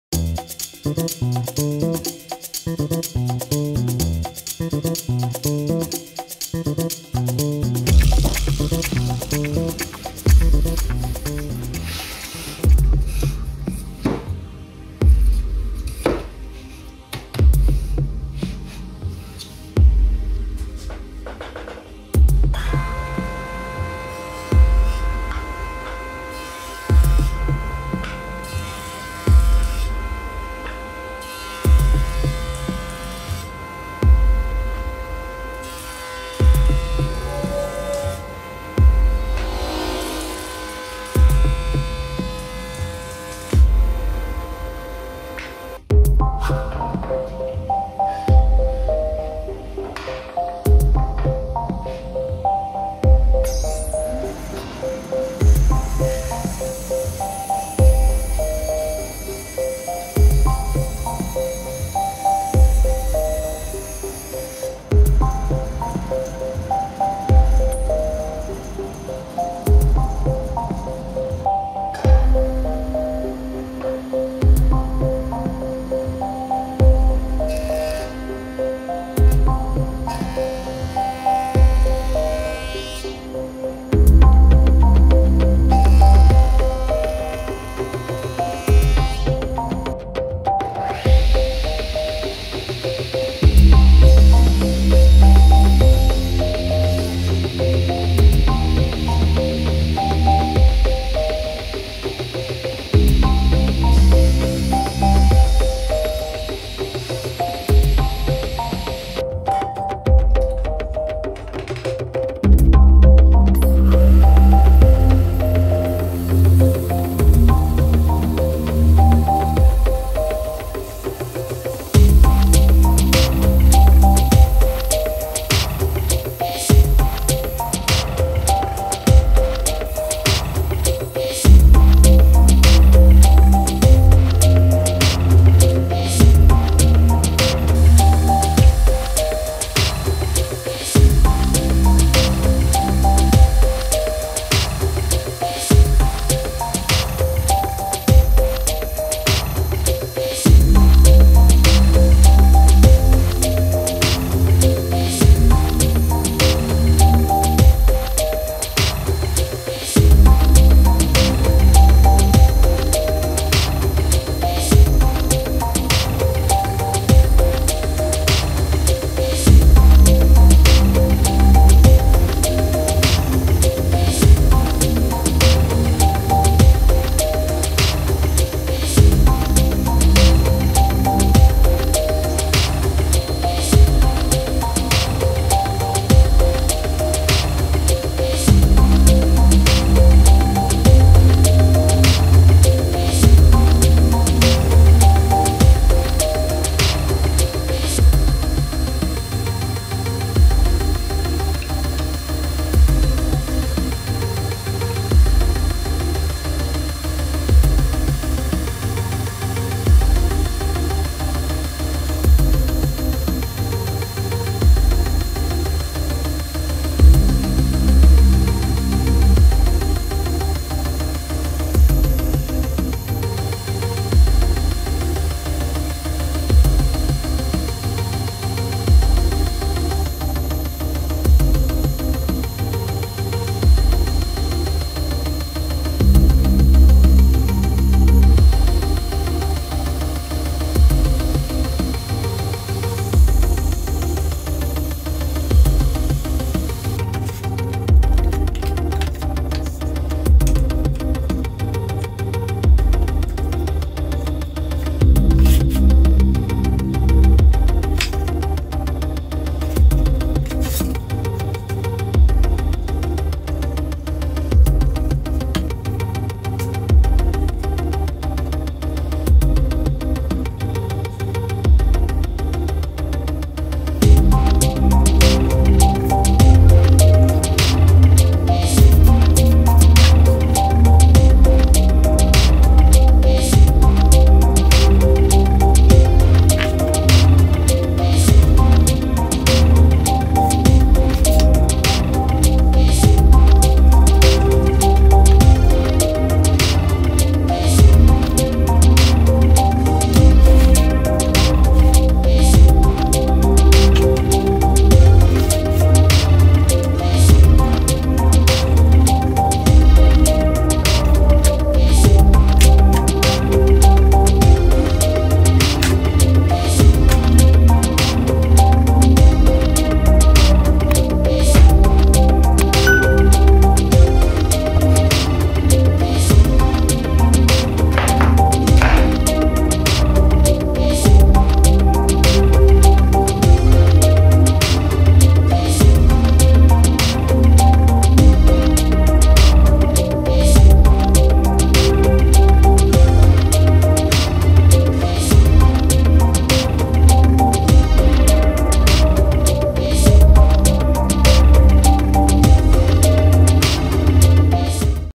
828. Rozsuwane domki z pniaka jesionu
Ostatnio przez naszą grupę dyskusyjną Domidrewno na Facebook regularnie przewijał się ten oto projekt. W mojej głowie od razu pojawił się pomysł jak go wykonać. Ruszyłem więc do warsztatu aby sprawdzić moją teorię ;) Jak mi poszło? Zobaczcie sami, zapraszam na kolejny film!

Nowe filmy Domidrewno w każdą środę, piątek i niedzielę o godzinie 17.00.

Domidrewno.pl - miejsce łączące pasjonatów pracy w drewnie!